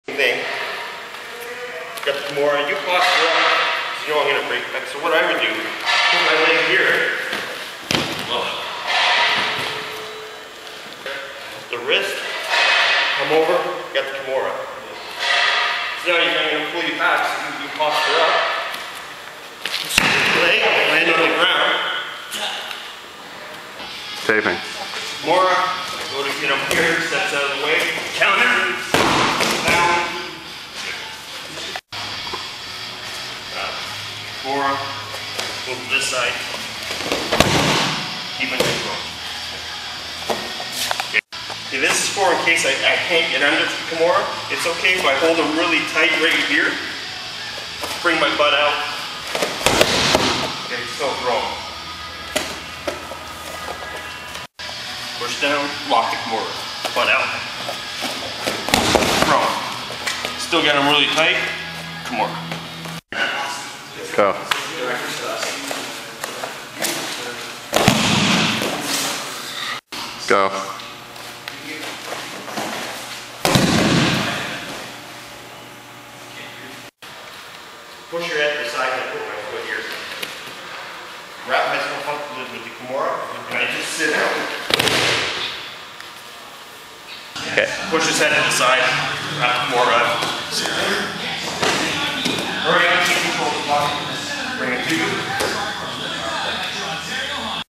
Double wrist lock take downs =Front, Back,Side and behind . Pain Peters Judo for MMA
Double wrist lock  counters and take downs Pain Peters @ no gi submission judo
Brampton Brampton Jiu Jitsu Judo MMA fighter
Pain Peters